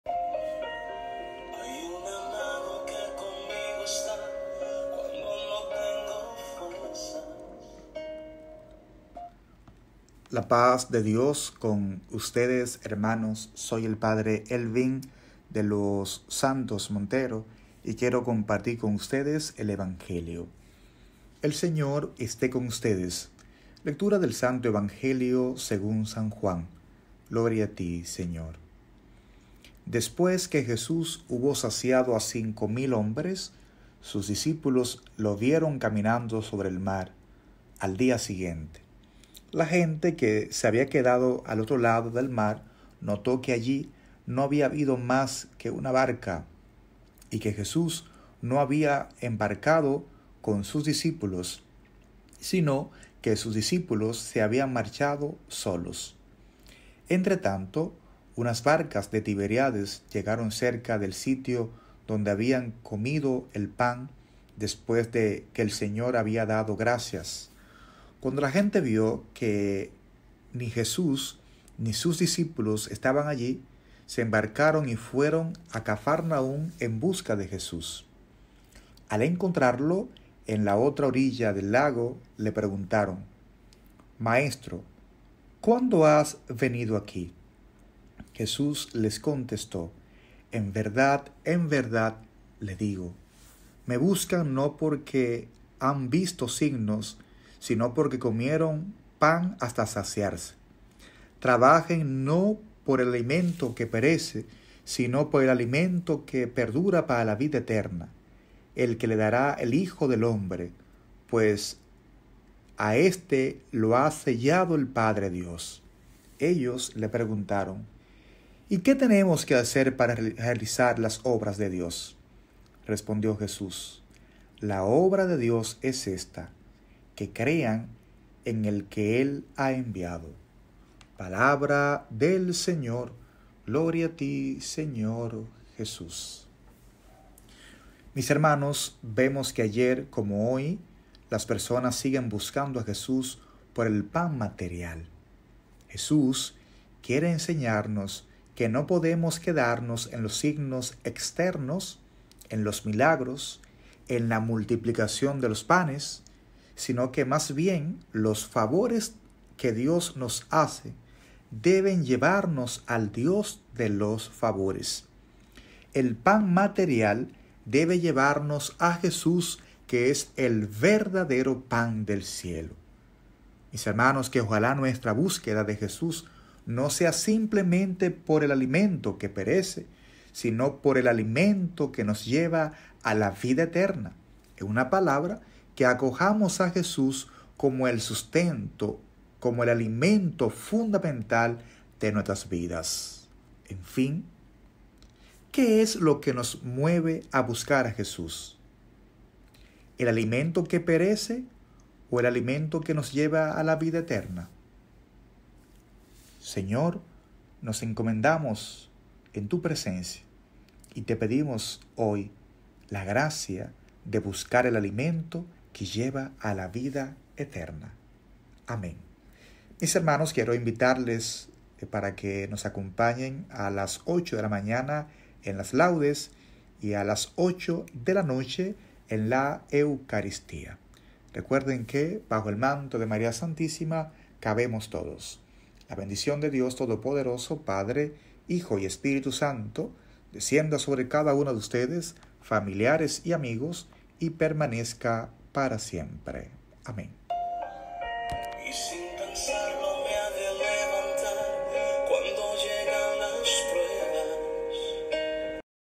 ¿Por qué tu buscas a Jesús?. P. Elvín De Los Santos Montero. Lunes 27 de Abril 2020.
Evangelio- Reflexión diaria.
Comunidad Bajo el Manto de María Santísima RD. (CBMMS RD)